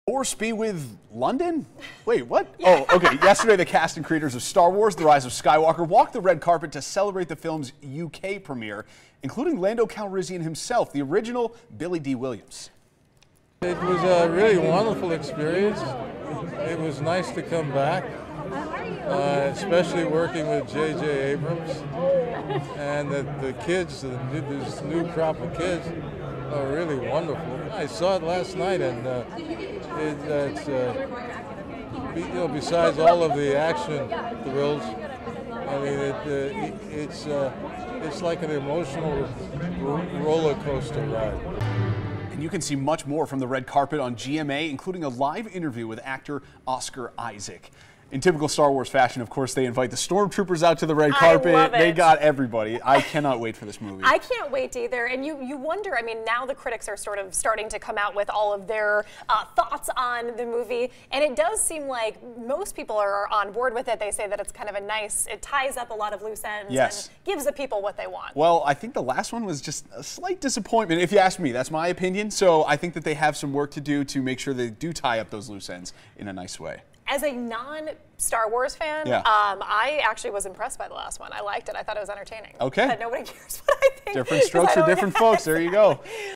London premiere of new Star Wars
"Star Wars: The Rise of Skywalker" premiered yesterday in London. Billy Dee Williams was at the premiere.